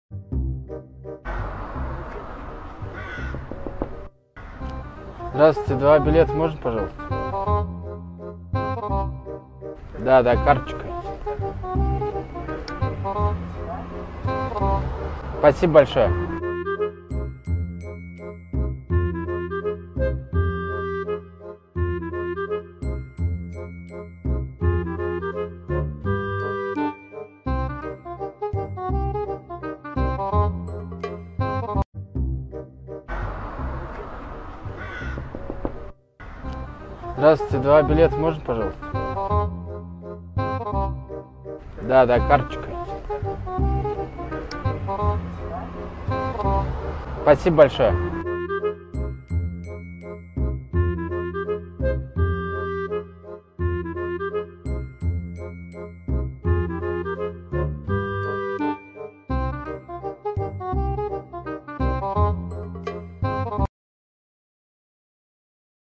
В Воскресенске появилась первая "умная" остановка, которая сама выдает билеты на автобус.
Бабушки до сих пор в шоке. 😊

Сделаем наш город лучше вместе. Освещаем все проблемы города без цензуры. Подробнее